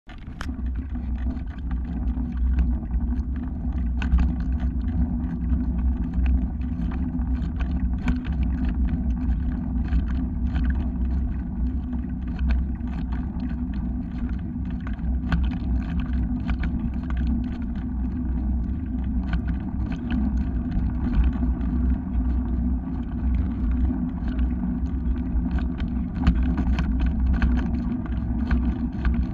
Goochmonster Videos: Riding the Landshark Ice Bike in Snow
GoPro video of riding the Landshark (aka: ice bike) up Paradise Road in the snow.
The Landshark is a single-speed with Paul's dropouts, equipped with a Cannondale Fatty, and studded mtb tires